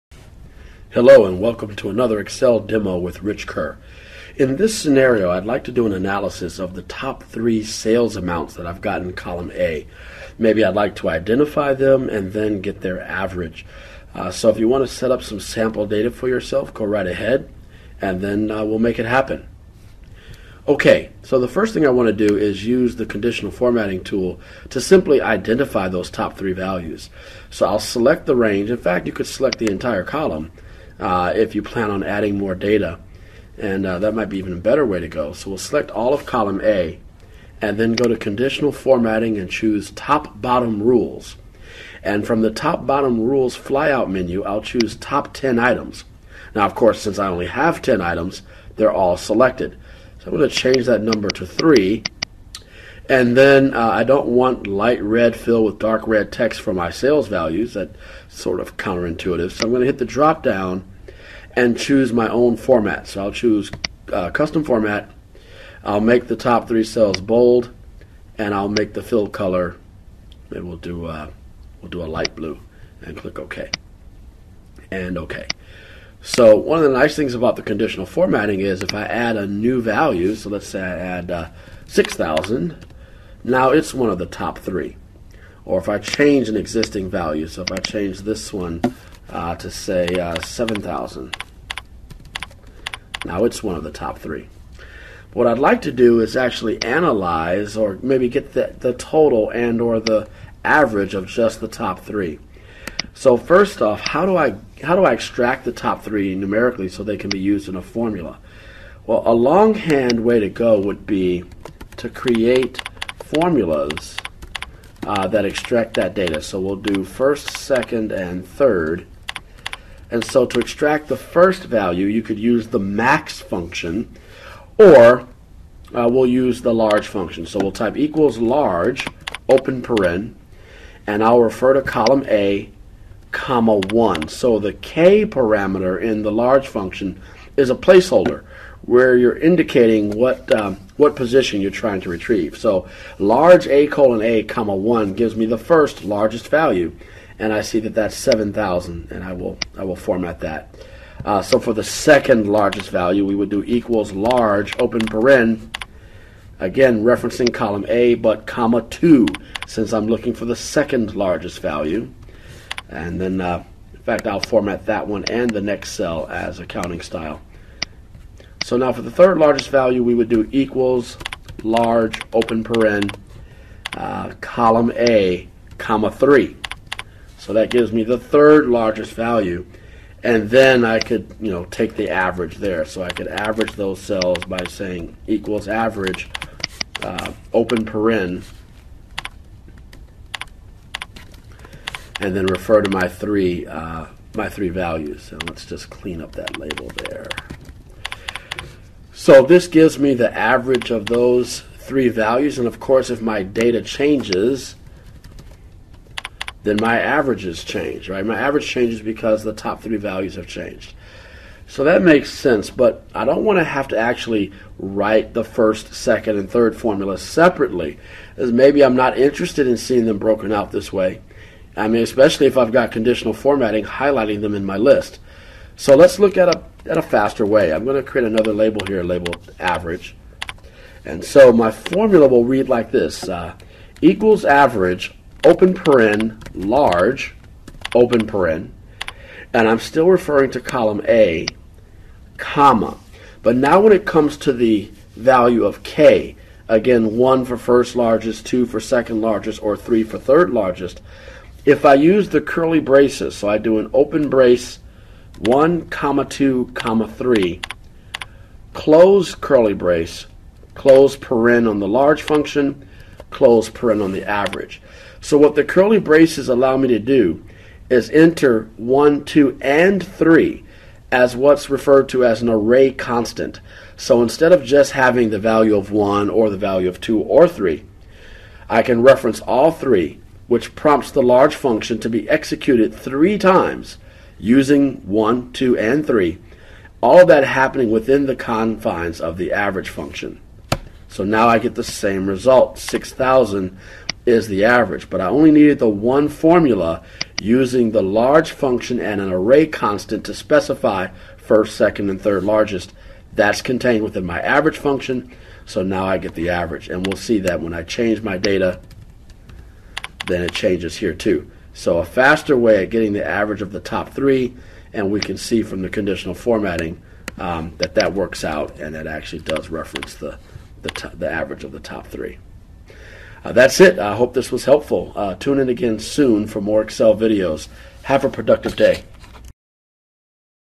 Excel Average of Top 3 Values
Use the LARGE function in an Array Constant to figure out the average of the top 3 values in a range.